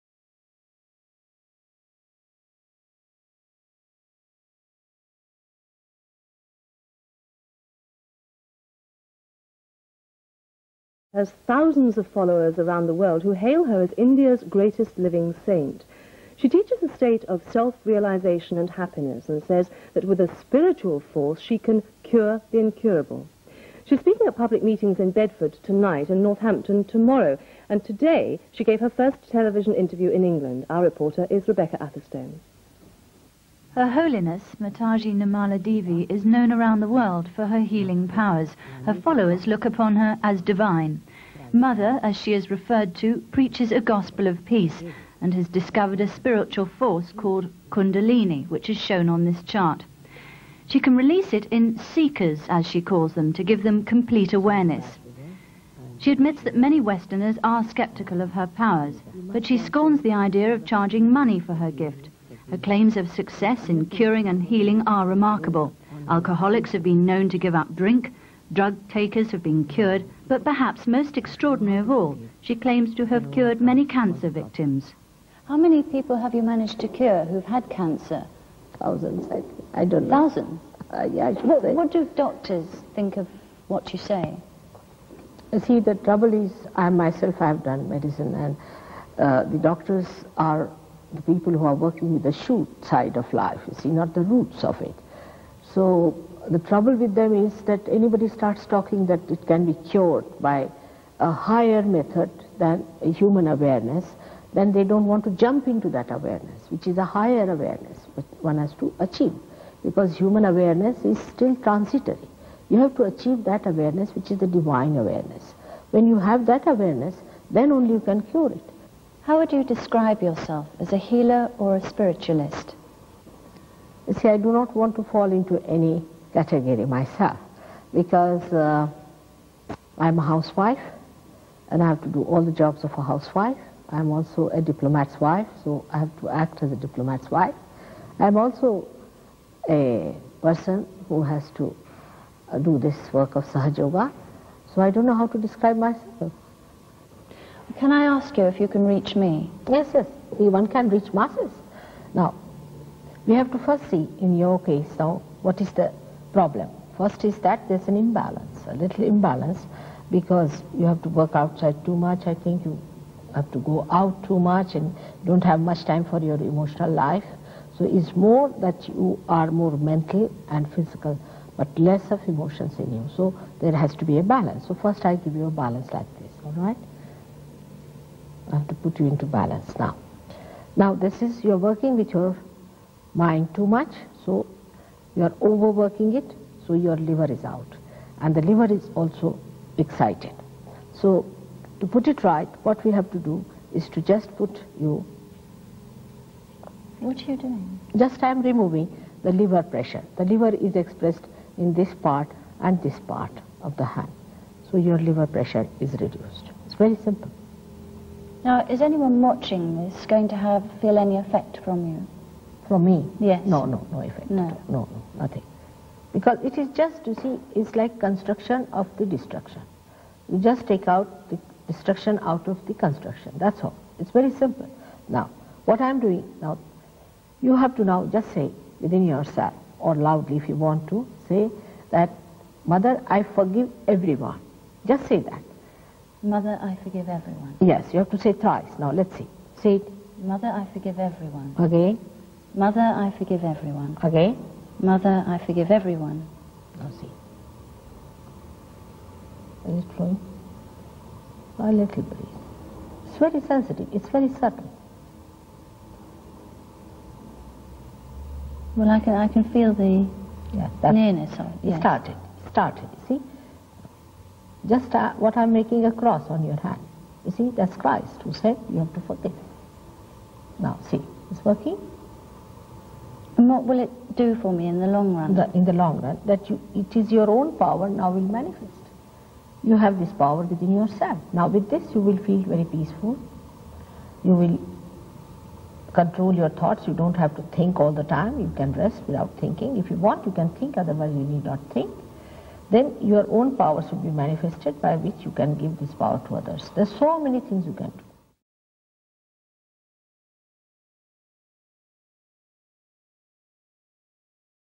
BBC TV Interview No 2 with Shri Mataji Nirmala Devi, founder of Sahaja Yoga meditation.
Early 1980's in Britain.